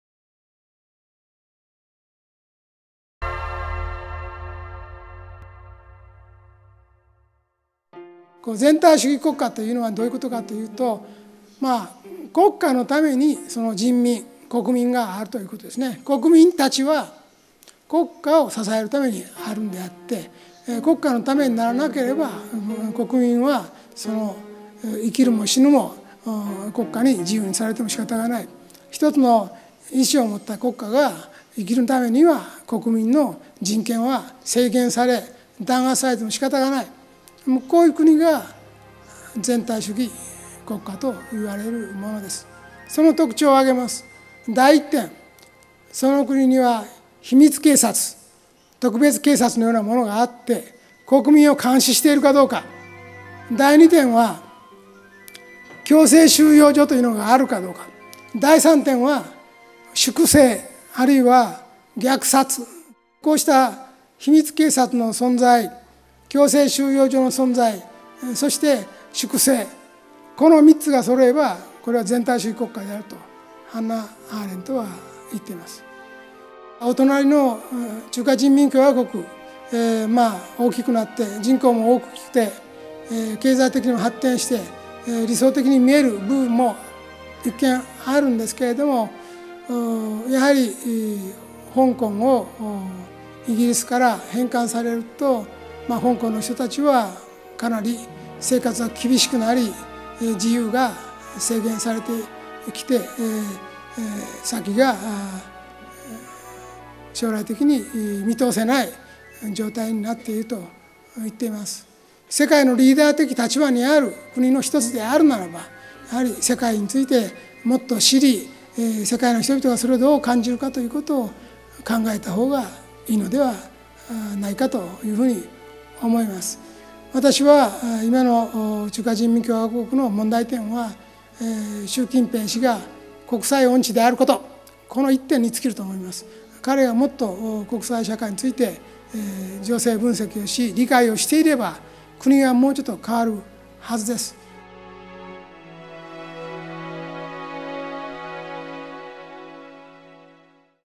Create Golden Age: Xi Jinping Has No Sense of International Relationships
"A country based on totalitarian principles says that the people—its citizens—exist for the sake of the country.

Totalitarian countries consider their people to be their servants, and when someone has not shown to serve his or her country, they are free to decide whether this person should live or perish. In order for a country of a united cause to survive, it is allowed to limit the rights of the people and suppress them.

This is what a totalitarian country is.

There are three signs of a totalitarian country.

The first sign is a secret or special police force keeping the country’s citizens under surveillance.

The second sign is the existence of internment camps.

The third sign is a system of purging or slaughtering people.

When a country is seen to have all three of these things—a secret police, internment camps, and a system of purging—this signifies a totalitarian country, according to Hannah Arendt.

Your neighboring country of the PRC, with its large population and developed economy, may seem an ideal place at first glance.

But since Hong Kong was reverted to the PRC, the Hong Kong people have faced difficult lives, suppressed freedom, and an uncertain future.

A leading country of the world should have more knowledge about the rest of the world and a better awareness of how it’s being perceived by the international society.

The problem in the PRC boils down to a single point, which is Xi Jinping’s lack of international sense.

His country should have seen more changes than it has thus far if Xi Jinping truly has a better knowledge and understanding of international society and its situation."